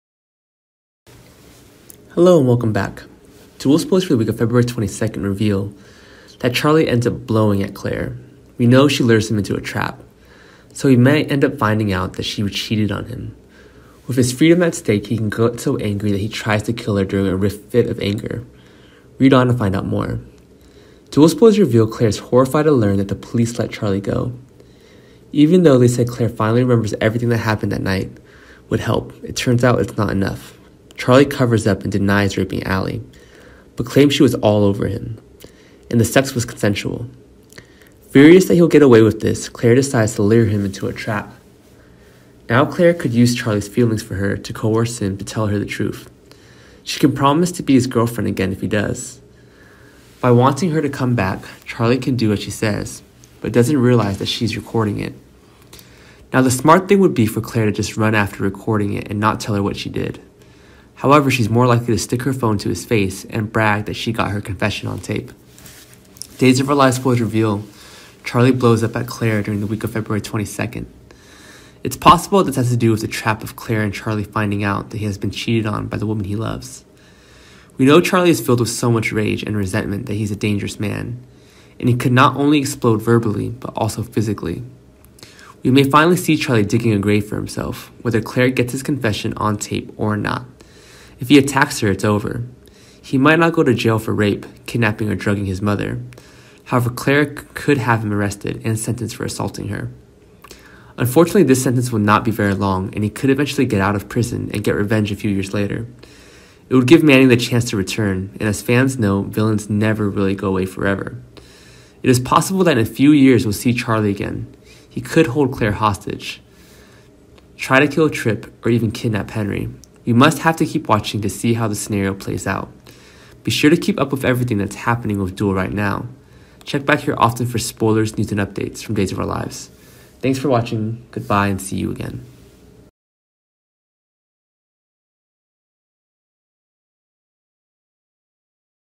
Days of our Lives Spoiler: Charlie kills Claire, even Salem acted indignantly
✔ Hello everyone.Days of our Lives Spoiler: Charlie kills Claire, even Salem acted indignantly